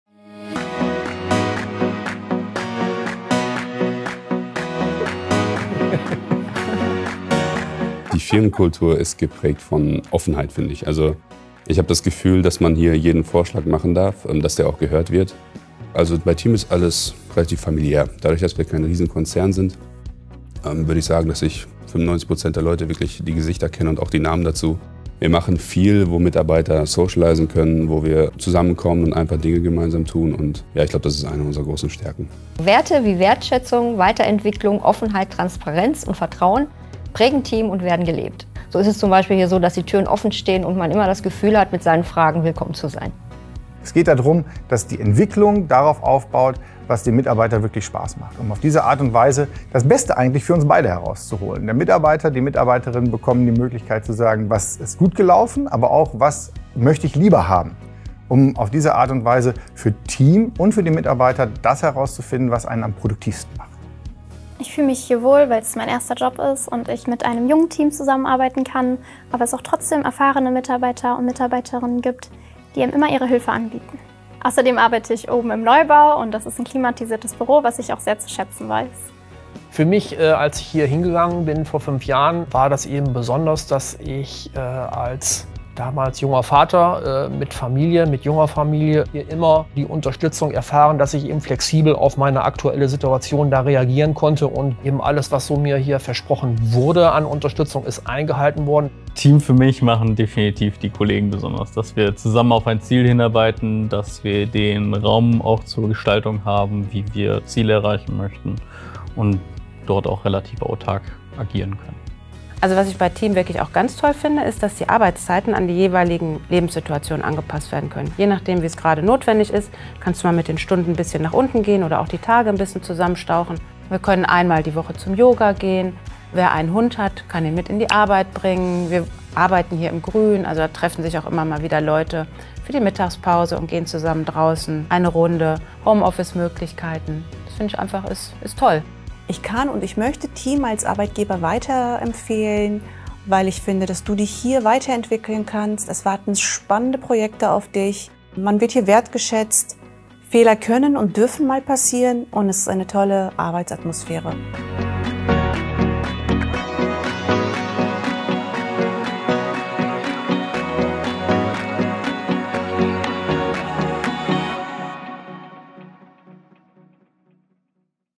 TEAM als Arbeitgeber | TEAM GmbH
Bei TEAM geht es nicht nur darum, seinen normalen Arbeitsalltag nachzugehen, sondern auch darum, eine echte Unternehmenskultur zu leben. Unsere Arbeitsatmosphäre ist geprägt  von Offenheit, Transparenz und gegenseitigem Respekt! Außerdem legen wir großen Wert auf eine gute Work-Life-Balance. ⚖️

Möchtet ihr mehr darüber erfahren, was uns ausmacht? Dann werft einen Blick in unser brandneues Video! 📽️

Wir sind stetig auf der Suche nach neuen Talenten in den Bereichen Softwareentwicklung, Consulting, Vertrieb und vielen mehr.

und finde die Position, die am besten zu dir passt.